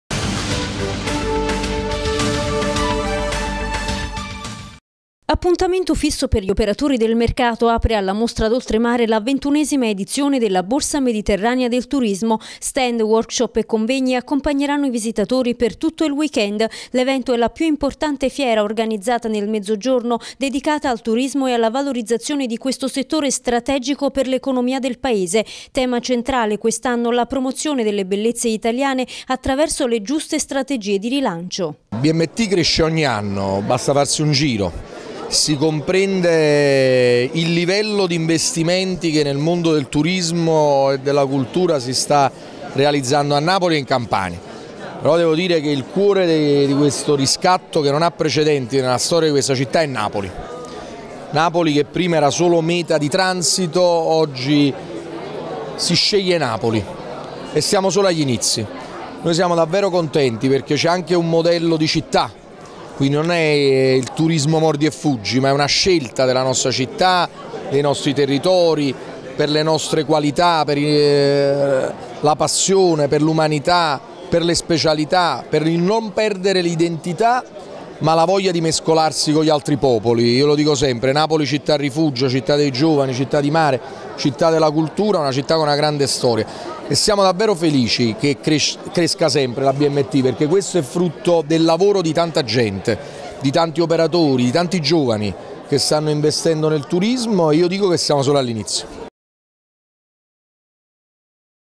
Al via la Borsa Mediterranea del Turismo
Alla Mostra d'Oltremare la XXI edizione della Borsa Mediterranea del Turismo.